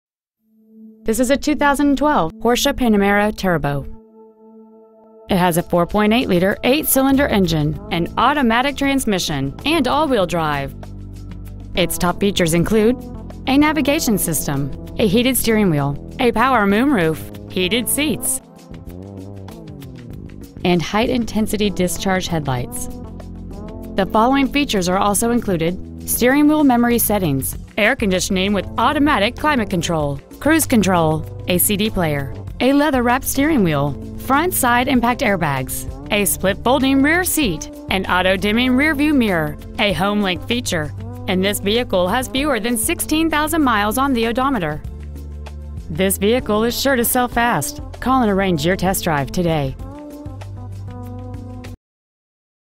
2012 Porsche Panamera Beverly Hills CA 90036
Year : 2012
 Make : Porsche
 Model : Panamera
 Engine : 4.8L V-8 cyl
 Trans . : Automatic
 Exterior : Black
 Miles : 15,960
 Interior : Black

 Used 2012 Porsche Panamera Beverly Hills CA